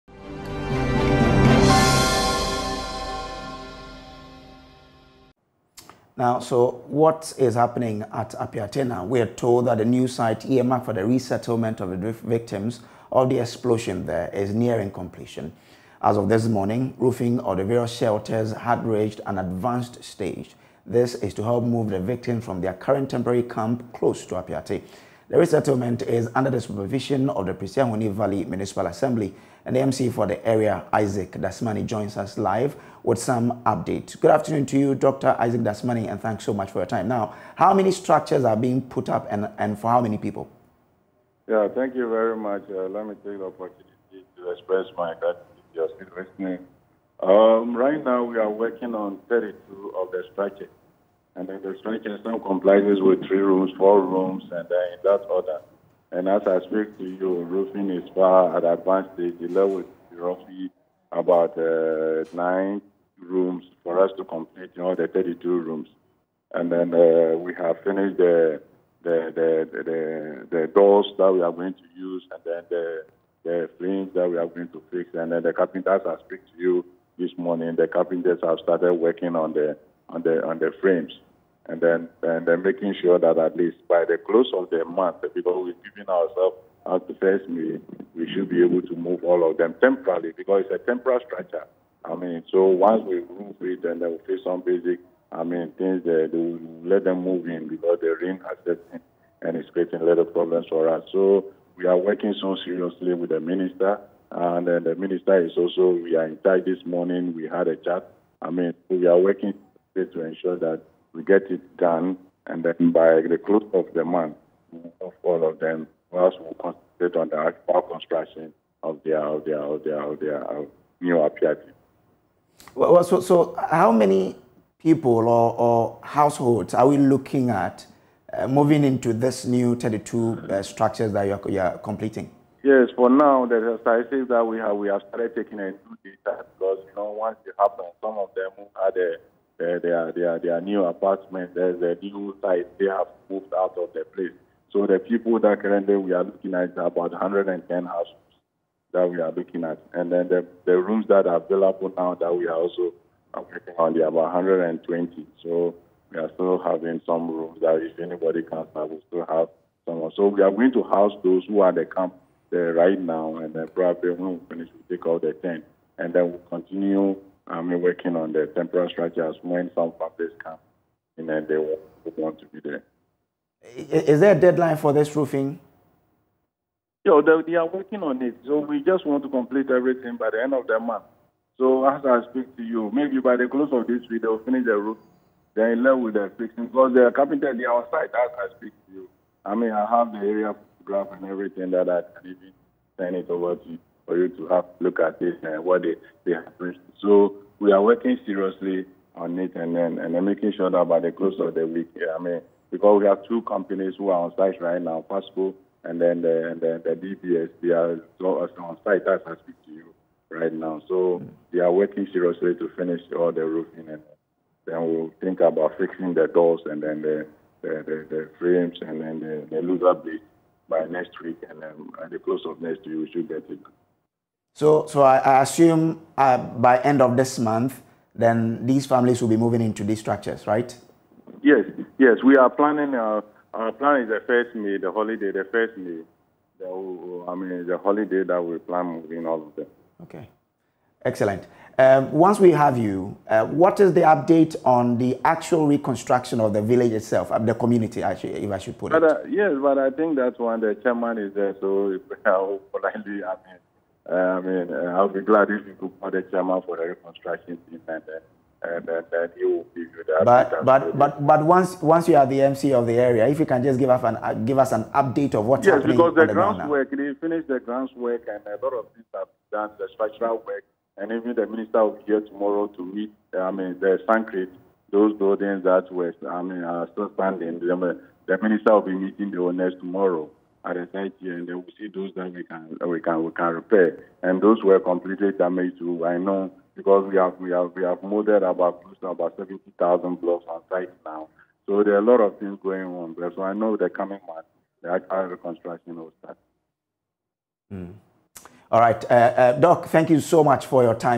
APPIATSE Explosion: 32 structures are currently being roofed – Dr. Dasmani - Joy News (20-4-22)
32 structures are currently being roofed – Dr. Dasmani - Joy News Today (20-4-22)